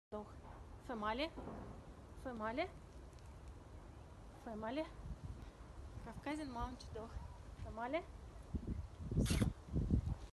Female Caucasian Shepherd # 02 for sale (litter #1)
Beautiful female for guarding and breeding for burning. There is a microchip. There are all vaccines. There is a pedigree. Delivery to any country.